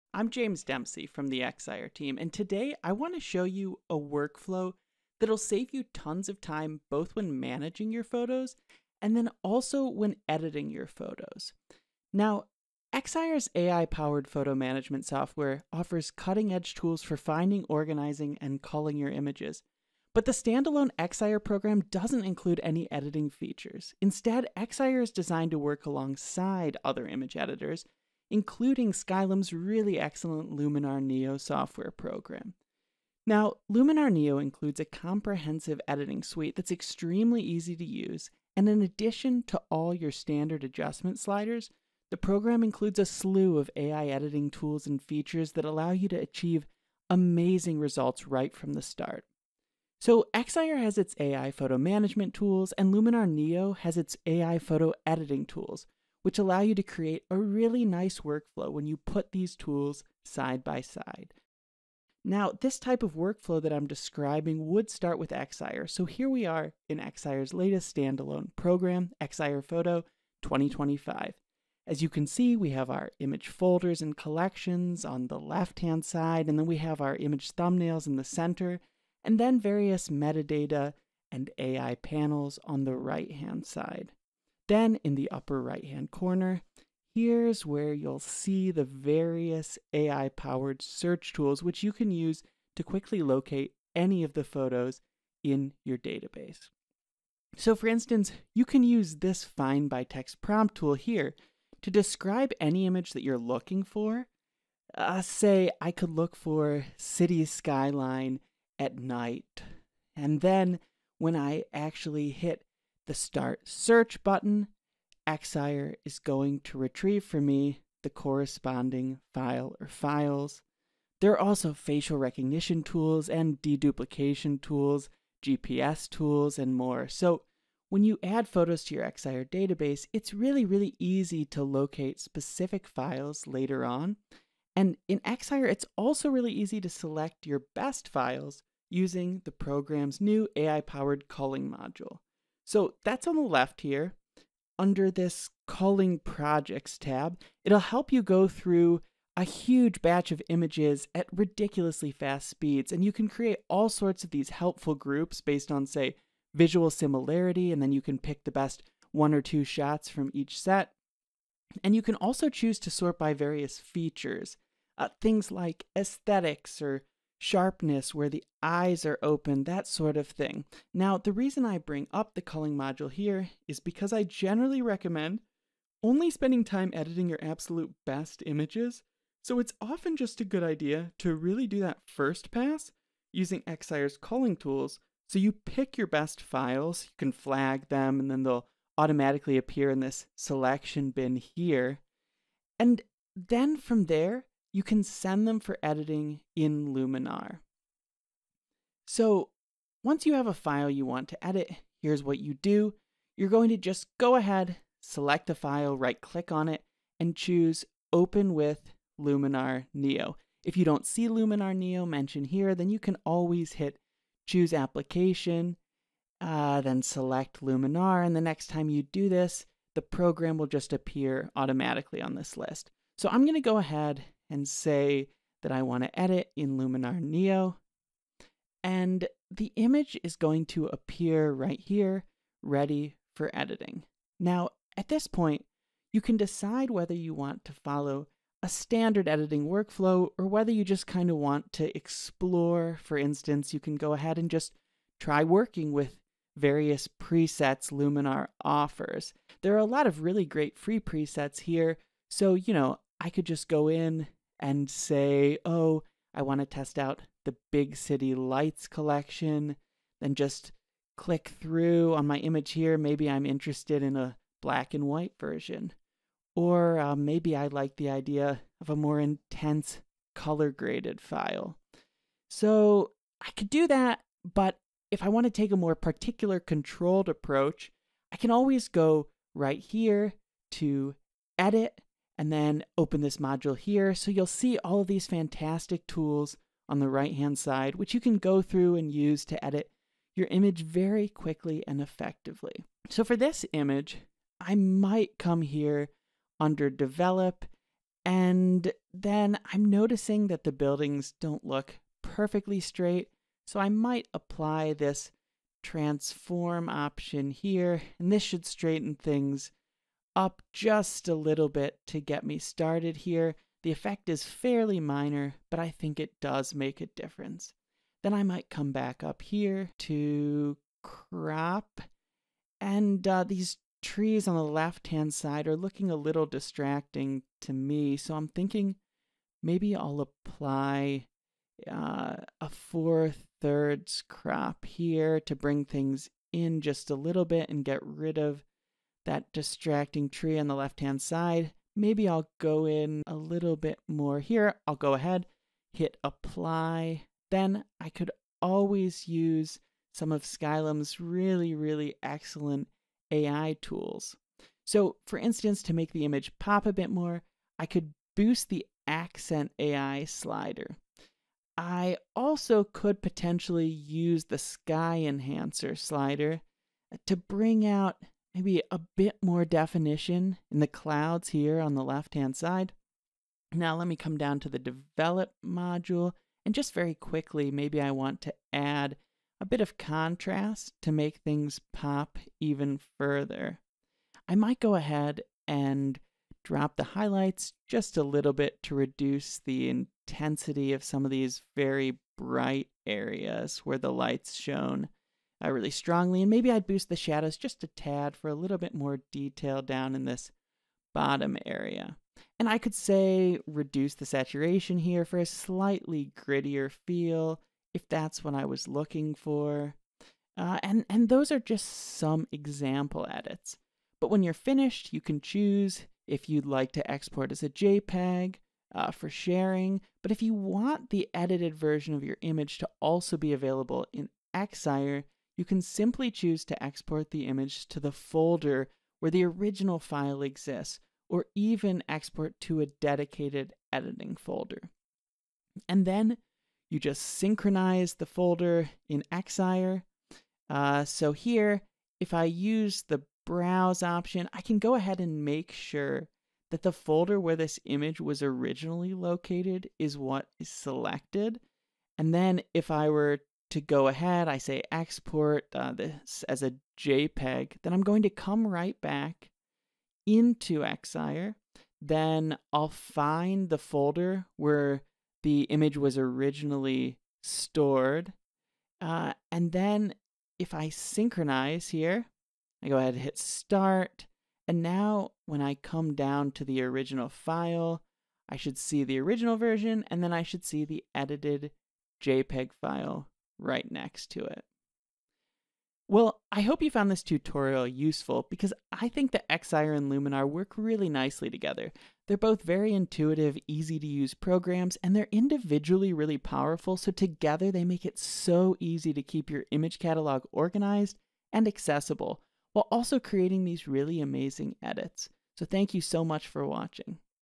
Amazing AI Workflow for Photographers (Excire Foto 2025 AND Luminar Neo)
Looking for a powerful workflow that combines Excire's AI-powered photo-management tools with Luminar Neo's AI-powered editing tools? Look no further!

In this tutorial, discover how to get organized with Excire and easily send photos off for editing with Luminar Neo - before bringing the edited versions right back into Excire!